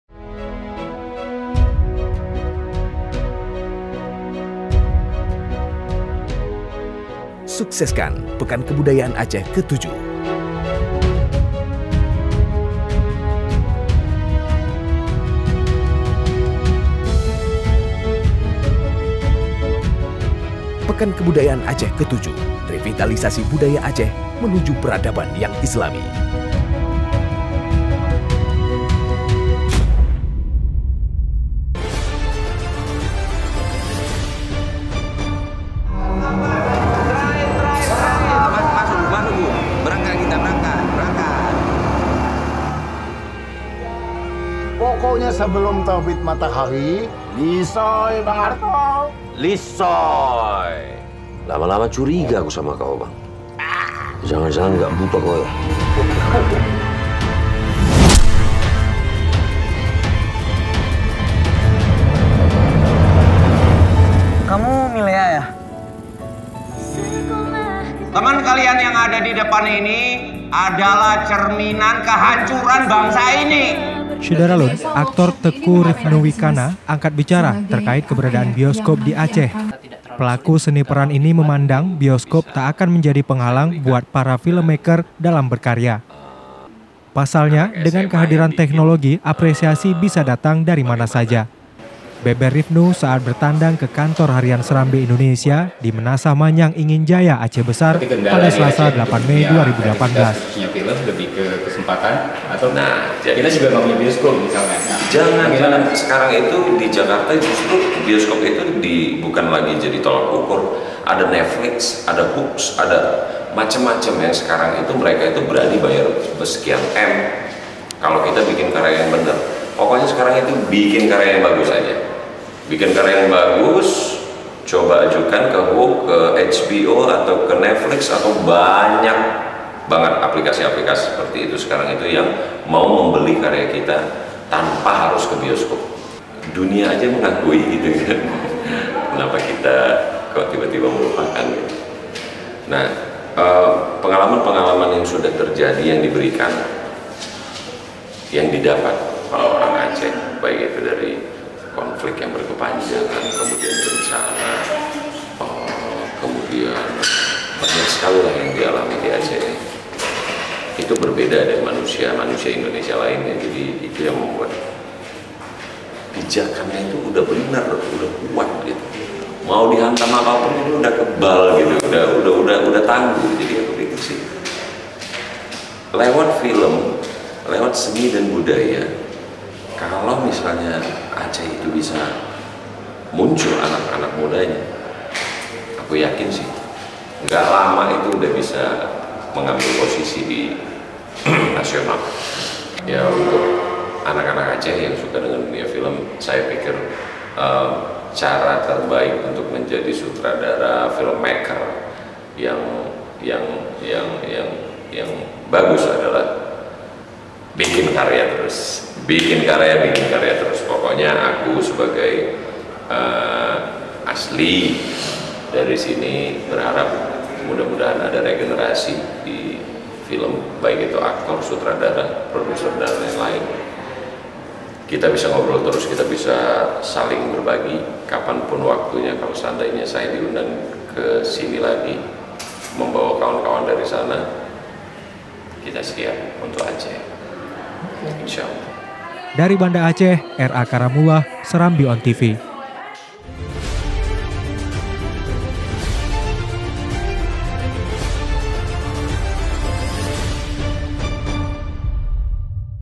Bioskop Tidak Perlu Untuk Aceh
SERAMBITV.COM, BANDA ACEH - Aktor, Teuku Rifnu Wikana angkat bicara terkait keberadaan bioskop di Aceh. Pelaku seni peran ini memandang, bioskop tak akan menjadi penghalang buat para filmmaker dalam berkarya. Pasalnya, dengan kehadiran teknologi, apresiasi bisa datang dari mana saja.

Beber Rifnu saat bertandang ke Kantor Harian Serambi Indonesia, Meunasah Manyang, Ingin Jaya, Aceh Besar, Selasa (8/5/2018).

Pemeran ‘Suripto’ dalam film ‘Dia Adalah Dilanku 1990’ ini diundang Badan Nasional Penanggulangan Teroris (BNPT) ke Aceh yang notabene adalah kampung halamannya untuk menjadi pemateri Workshop kepada para pelajar SMA se-Kota Banda Aceh.

Menurut nominasi aktor pembantu terbaik lewat film ‘Darah Garuda, Karakteristik orang Aceh yang tangguh menjadi pijakan yang kuat. Kalau lewat seni dan budaya anak-anak muda Aceh bisa muncul, ia optimis mereka akan mengambi bagian di pentas nasional.